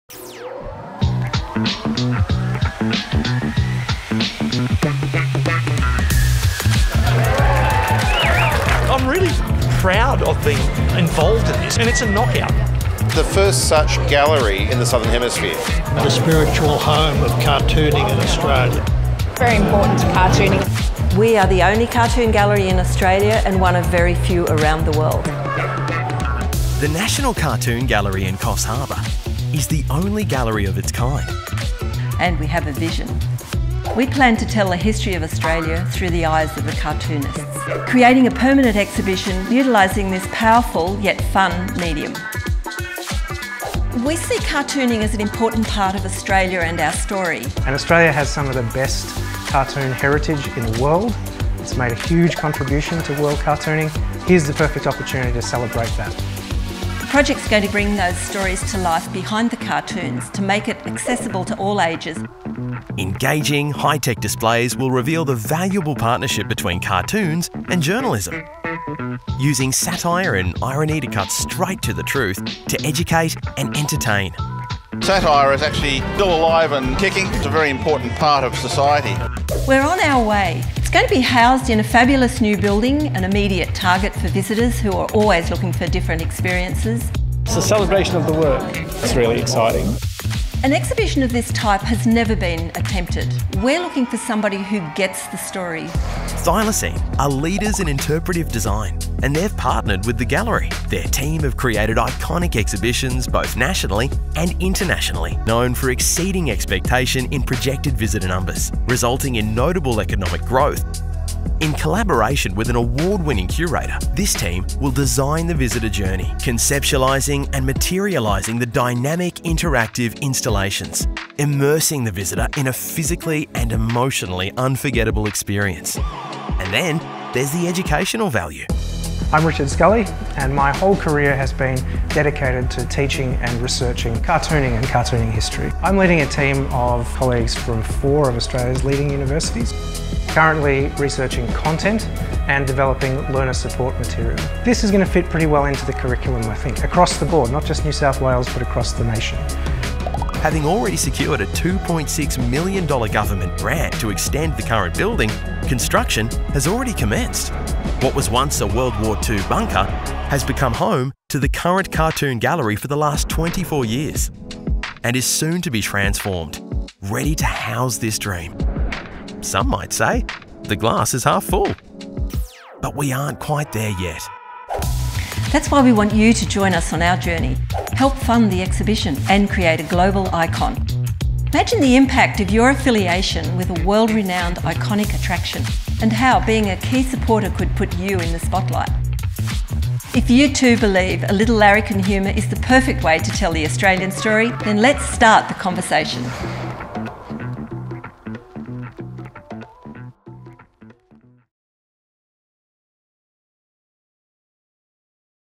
National Cartoon Gallery Sponsorship
A National Vision
At the National Cartoon Gallery in Coffs Harbour we see something truly unique emerging on the Australian cultural landscape.   Once a ‘hidden gem’ on the Coffs Coast landscape, known as The Bunker, the National Cartoon Gallery is literally ‘rising from the ground’ with a magnificent new gallery currently under construction.
What do we need to complete the Vision?
We need a funding partners to take us to the finishing line.

Margaret Cameron, Manager - 0418 708

Paul McKeon, Chairman - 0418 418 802